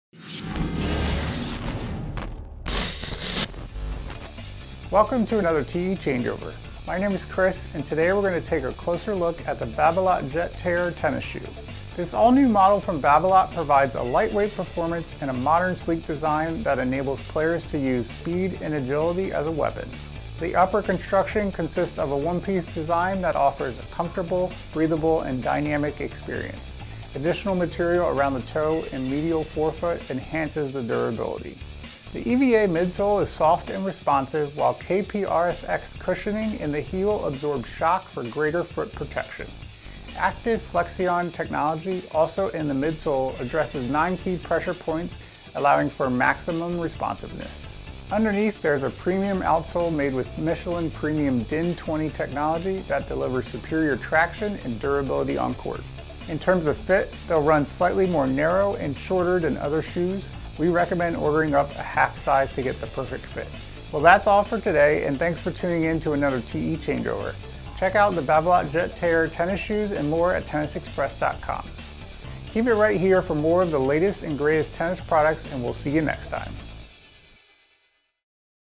Babalot Jet Tere Shoe Preview | Tennis Express
The Babolat Jet Tere All Court Tennis Shoes are designed to provide lightweight performance without compromise on comfort. Similar to other Babolat shoes, Active Flexion technology in the Jet Tere addresses 9 key pressure points for additional responsiveness while KPRS-X provides streamlined cushioning. These shoes represent a great option for tennis players seeking greater agility, and more dynamic quickness.